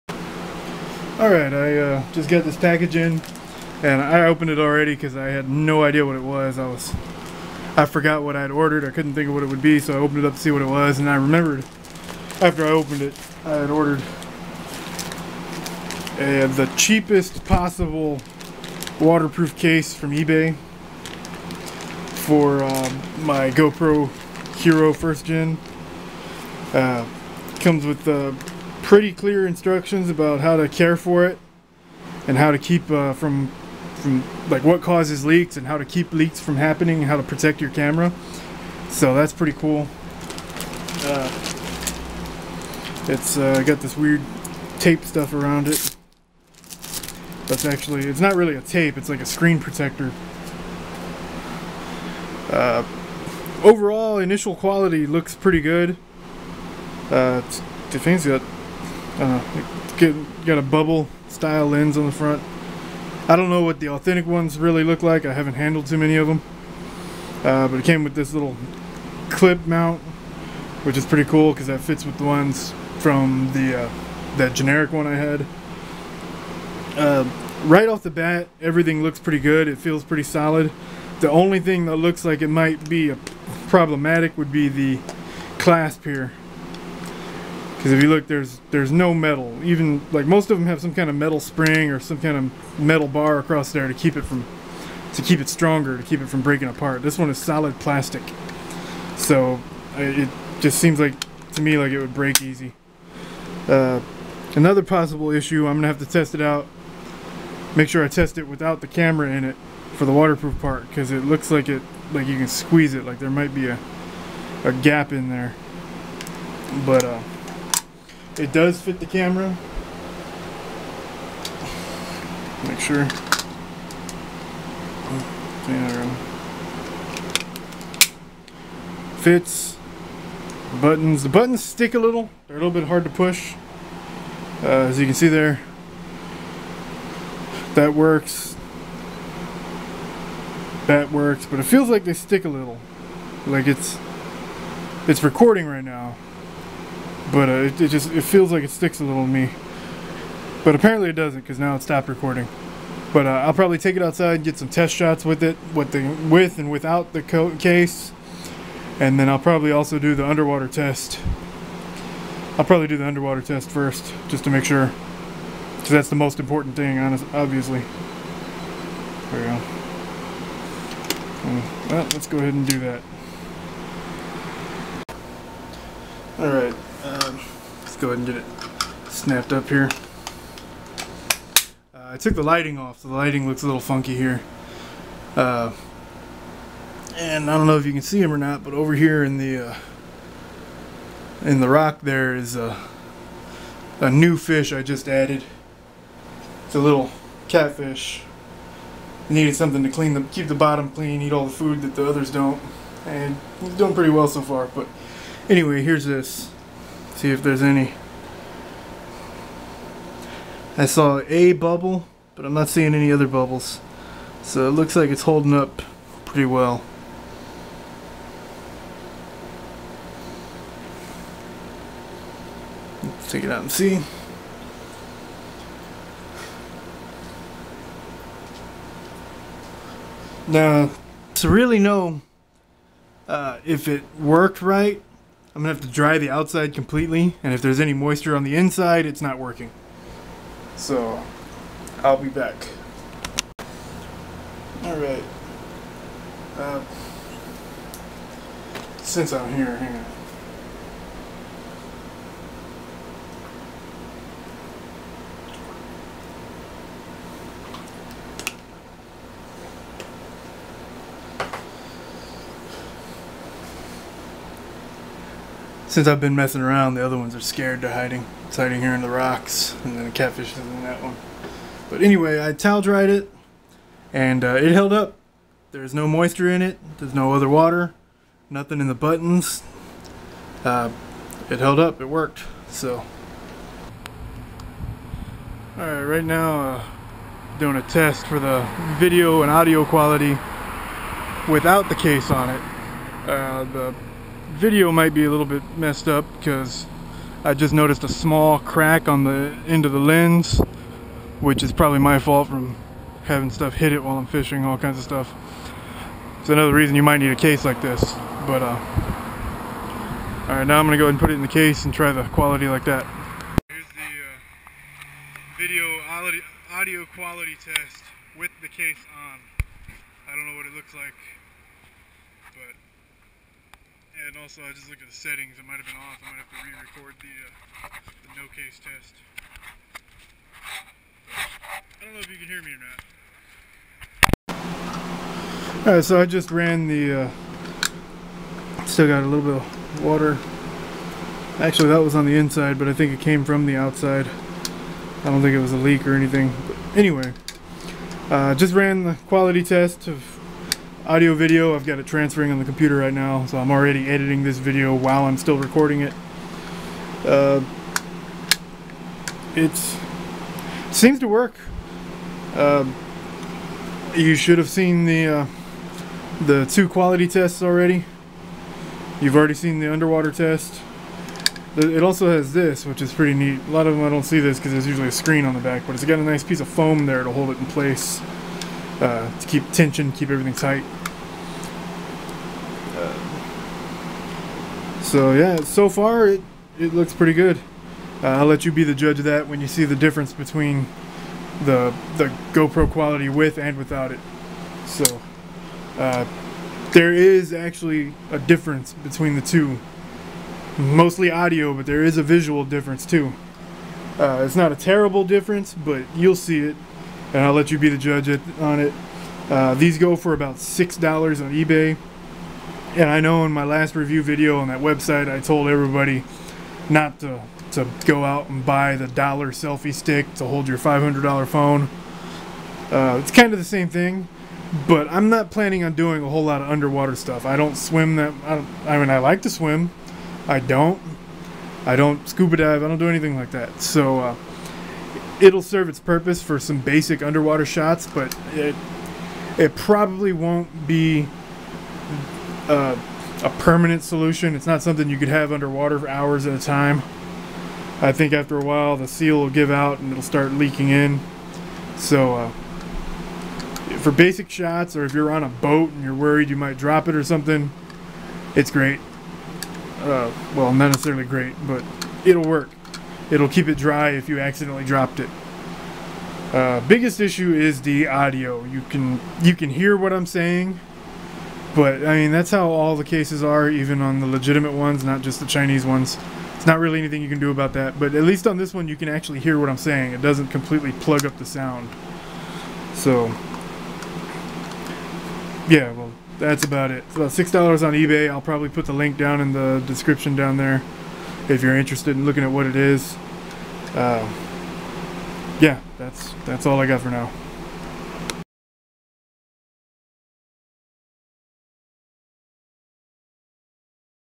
Unboxing Cheapest GoPro Waterproof Housing Review - Worth the Risk?
It seemed to get the job done on this quick test, but I wouldn't want to risk an expensive camera using this on your next scuba adventure. Also, there's some weird choppy pixelation in this video, it was caused by the editing software, and I redid this video 4 times trying to fix it. This was the best version.

eBay listing